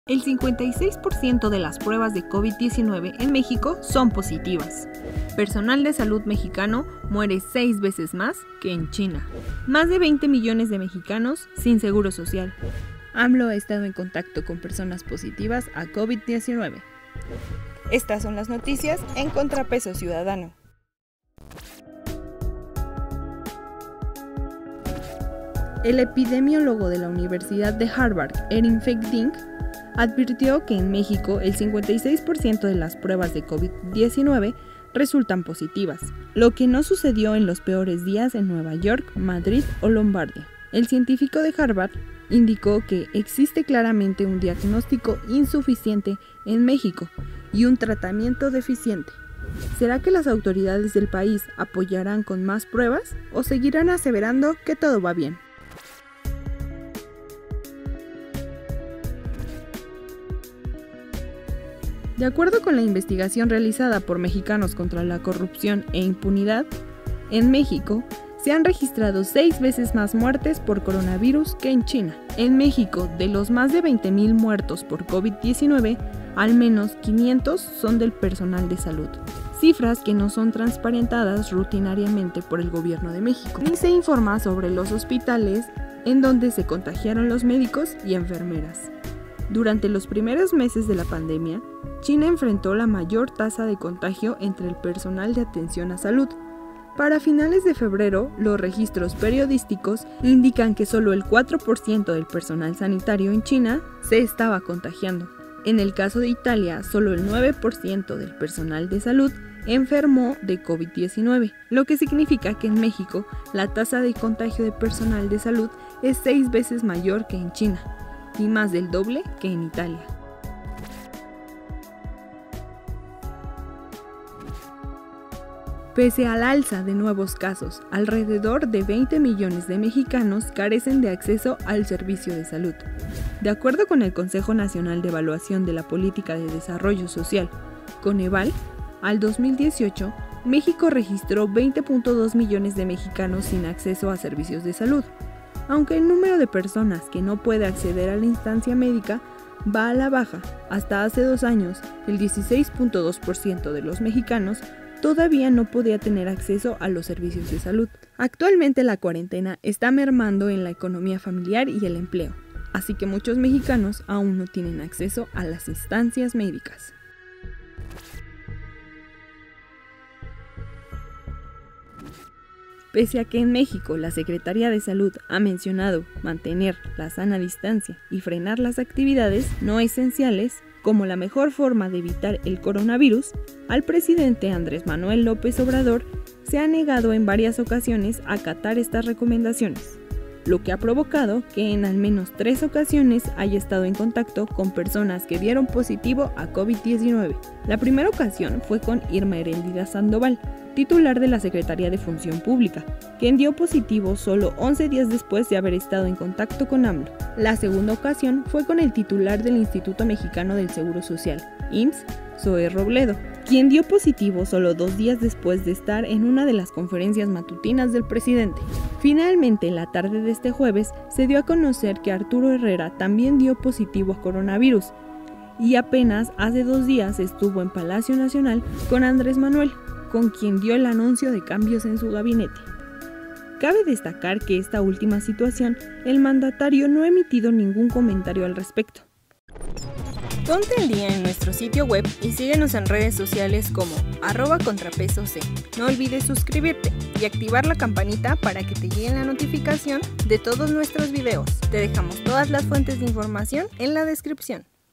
AMLO ha estado en contacto con personas positivas a Covid-19 | Top de Noticias
Pese a que en México la secretaría de Salud ha mencionado que mantener la sana distancia y frenar las actividades no esenciales es la mejor forma de evitar al coronavirus Covid-19, el presidente Andrés Manuel López Obrador se ha negado en varias ocasiones.

FUENTES DE IN FORMACIÓN
ESTOY LLORANDO POR MÉXICO”, EL 56% DE PRUEBAS DE COVID-19 SON POSITIVAS: EPIDEMIÓLOGO

PERSONAL DE SALUD DE MÉXICO MUERE 6 VECES MÁS QUE EN CHINA: MCCI

COVID-19 AL ALZA Y MÁS DE 20 MILLONES DE MEXICANOS CARECEN DE ACCESO A SERVICIOS DE SALUD

3 VECES EN LAS QUE AMLO TUVO CONTACTO CON PERSONAS CON COVID-+
19